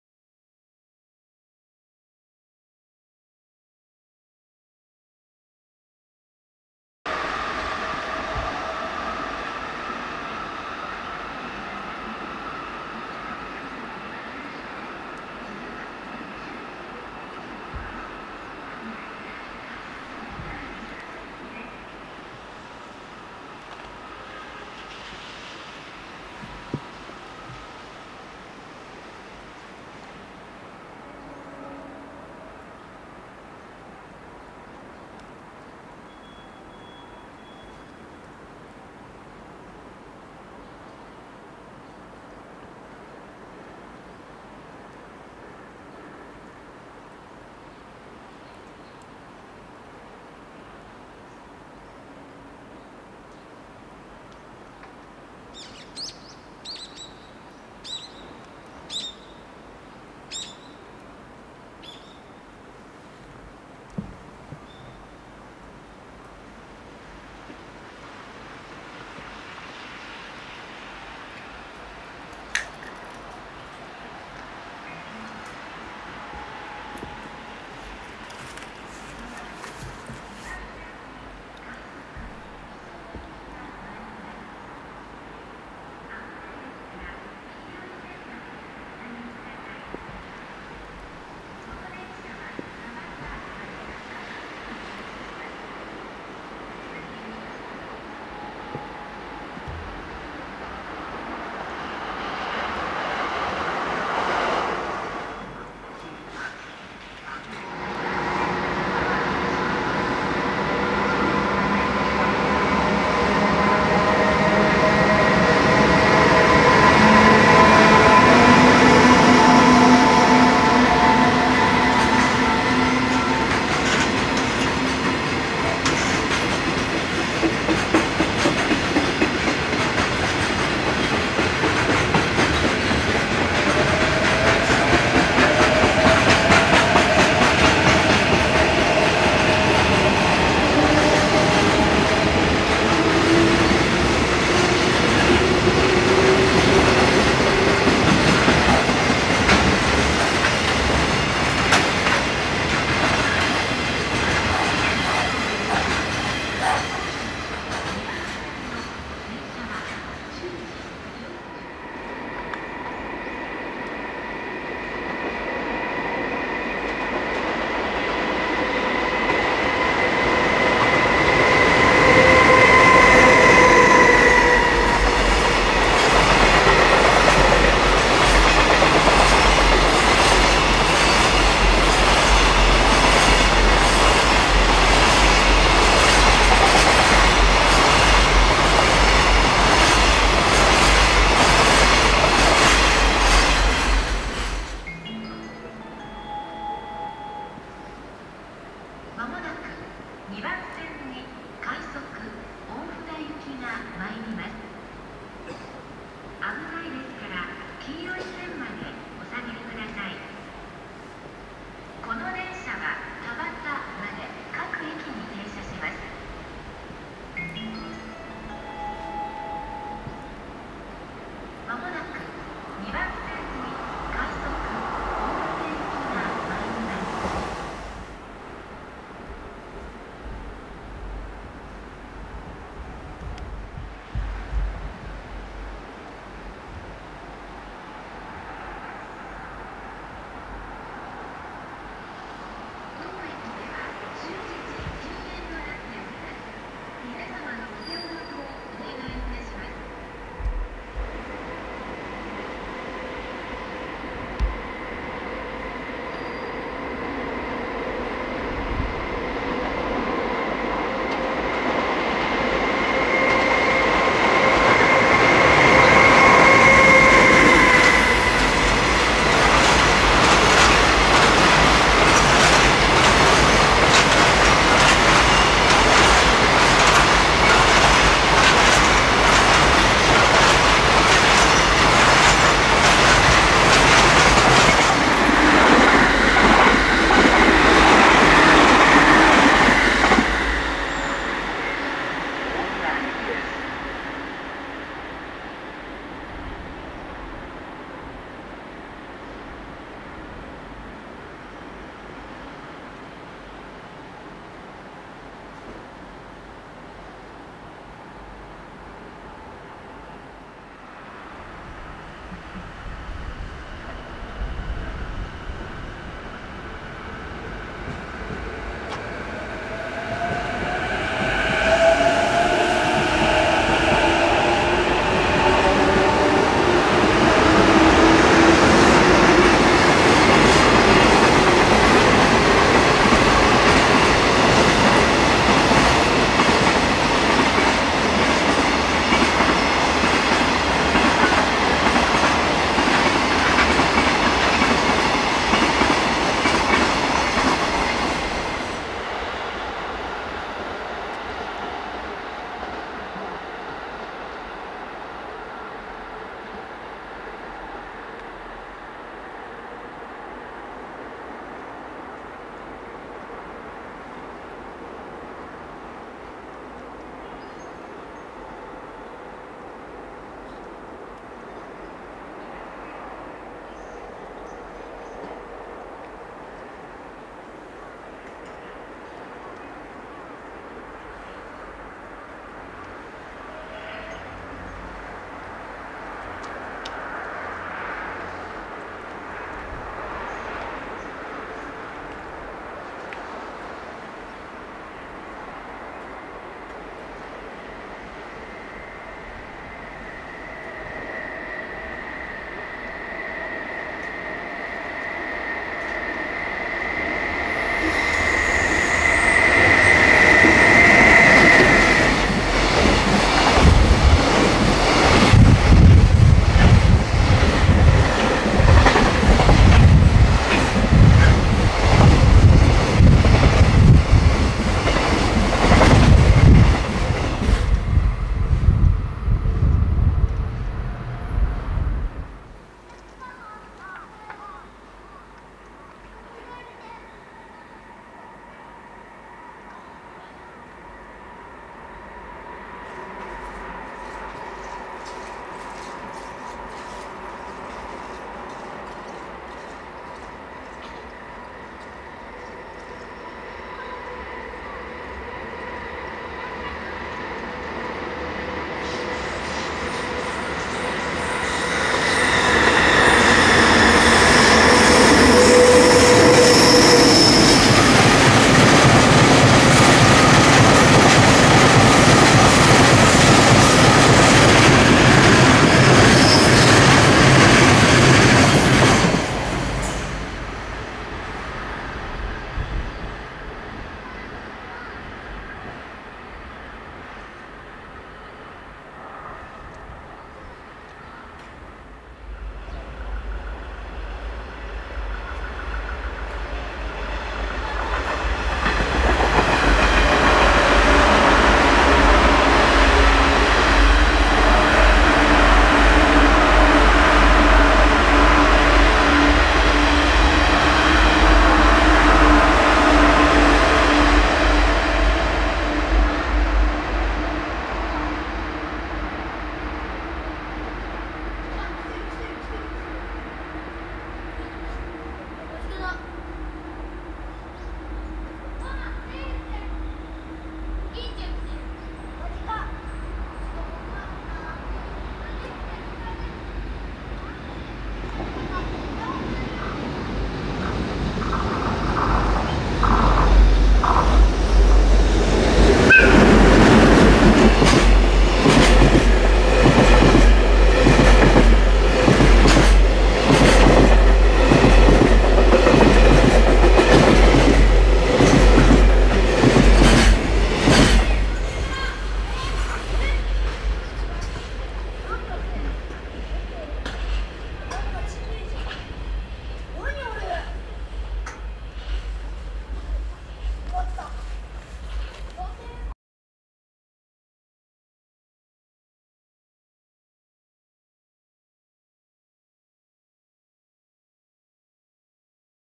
ダイヤ改正後、昼間のEH500牽引列車を与野近辺で撮影 2014/03/27
与野駅、土呂駅、北浦和～与野間にて。２０１４/３ダイヤ改正後の金太郎が牽く貨物列車を撮影しました。３０７４レから２０７３ﾚまでです。